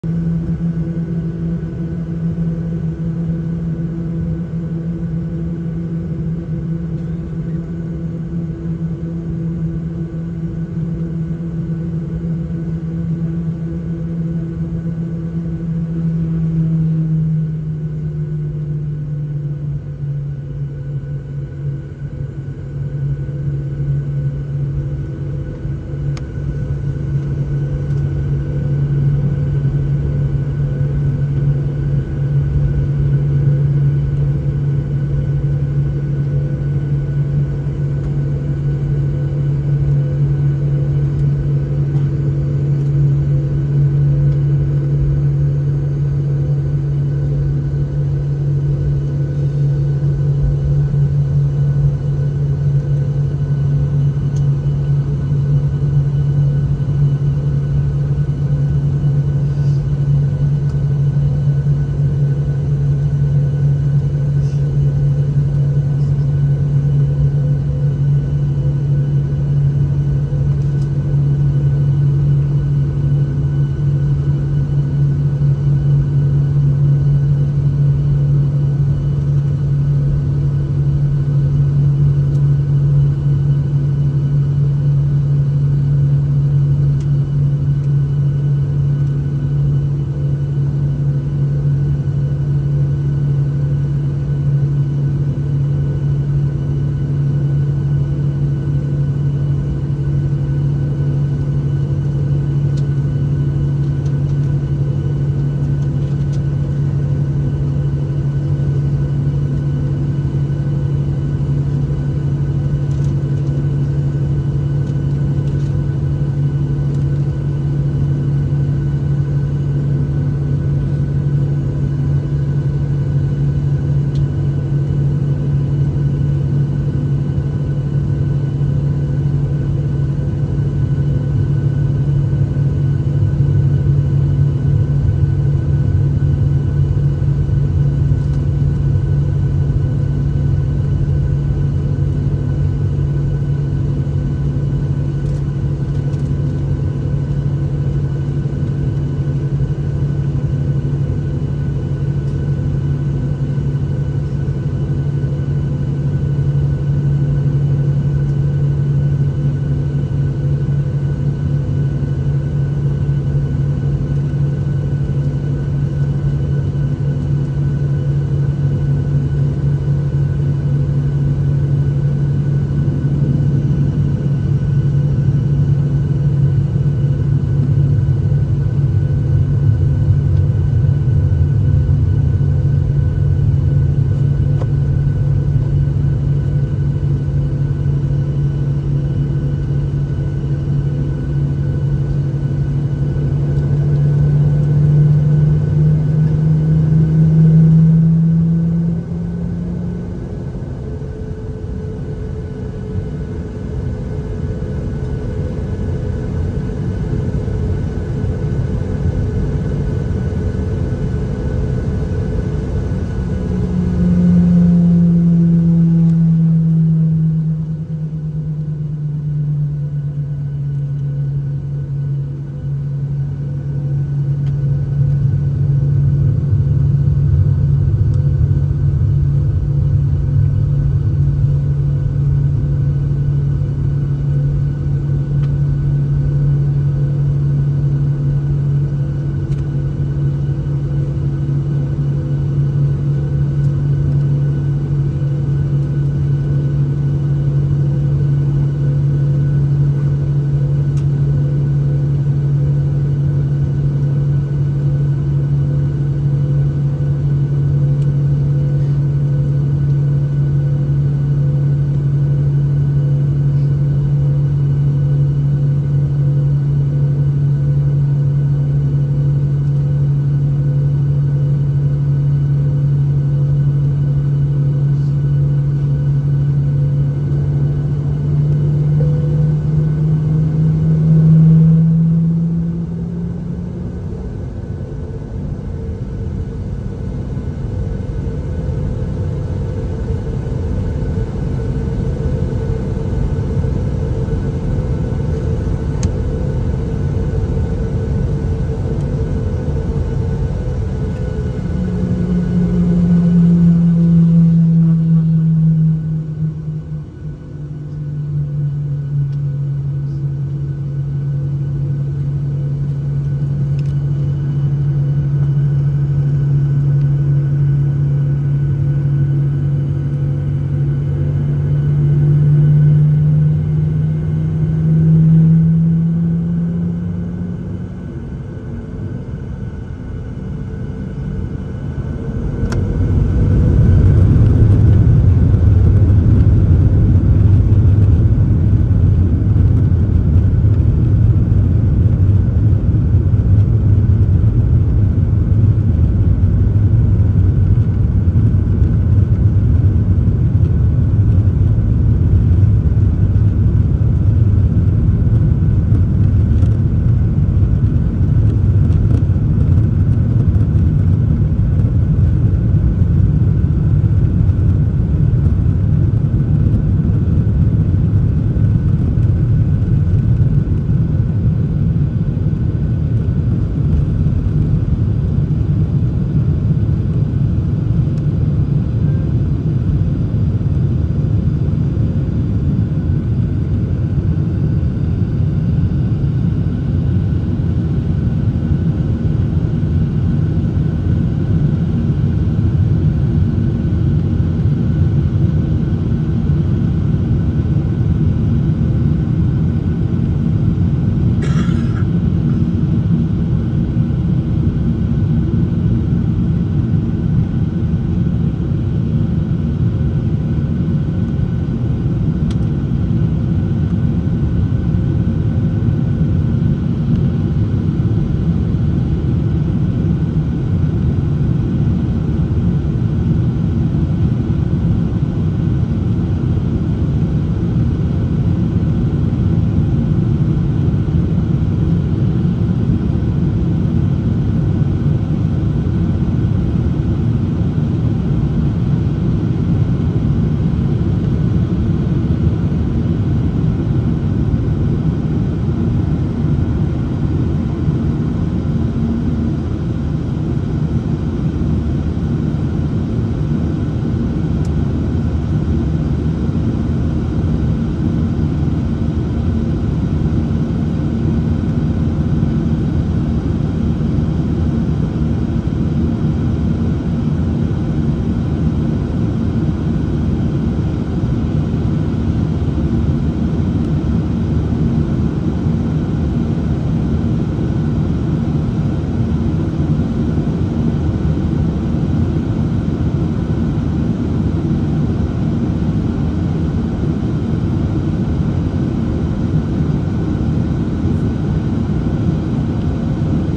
Dallas Airport Takeoff (DFW) - 1/21/24
Note the 787 taking off a few planes before us and an Avianca jet as well as a tandem takeoff out the window.